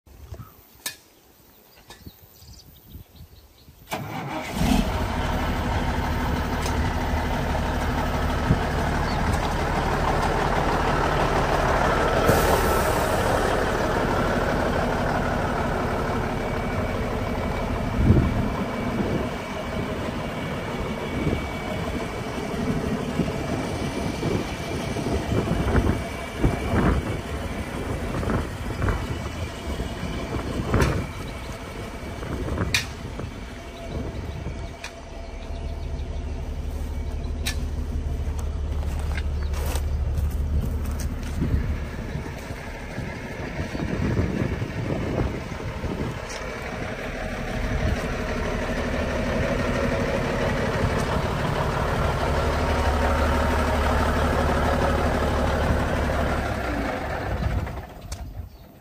1994 FREIGHTLINER FLD120 For Sale
1994 Freightliner FLD120 Aluminum Dump Truck, 1,203,439 Miles Showing but has a new motor put in, 11R24.5 Steers, 11R24.5 Drives, 295/75R22.5 Tag Axle, Aluminum Wheels,13 Speed Manual Transmission, Heat, Air, Radio, Air Ride Drivers Seat, Wet Kit, Scott LL2000 Dual Cylinder Hoist, 4 Rear Air Bag Suspension, Air lift Rear Tag Axle, 100 Gal Aluminum Fuel Tank, 700 Bushel Box, 20'x8'x5', Electric Tarp, Truck runs, drives, and operates like it should! Very handy!